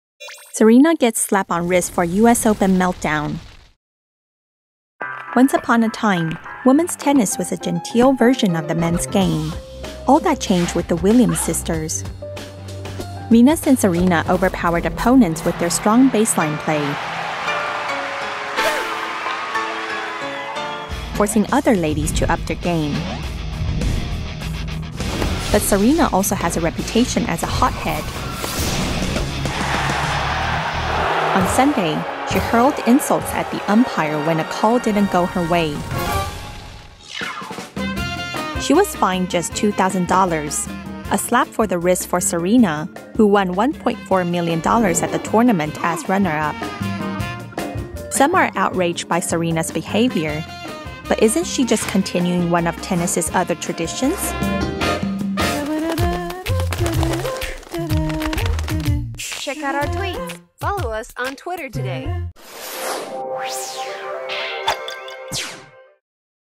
Serena Williams gets slap on wrist for US Open meltdown
Once upon a time, women's tennis was a genteel version of the men's game. All that changed with the Williams sisters.

Venus and Serena overpowered opponents with their strong baseline play, forcing other ladies to up their game.

But Serena also has a reputation as a hothead.

On Sunday, she hurled insults at the umpire when a call didn't go her way.

She was fined just $2,000. A slap for the wrist for Serena, who won $1.4 million at the tournament as runner-up.

Some are outraged by Serena's behavior. But isn't she just continuing one of tennis' other traditions?